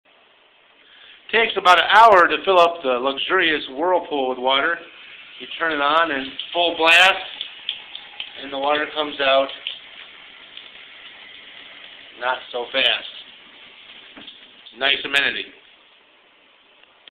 Jacuzzi
The address says it all: Wooded ravine lot with frontage on small private lake. Solid, limestone executive ranch with all of the upgrades. 4 bedroom, 2.5 bath. Stainless steel appliances, new kitchen & baths, master bath w/European glass shower & marble surround jacuzzi, 2 working limestone fireplaces, new rec room in walkout, great entertaining deck off of dining area. 2 car attached garage. A paved driveway leads you from a quiet neighborhood into your own peaceful setting. $1275/ month, $1275 security deposit. Tenants pay all utilities. Call  Heritage Leasing to arrange your showing.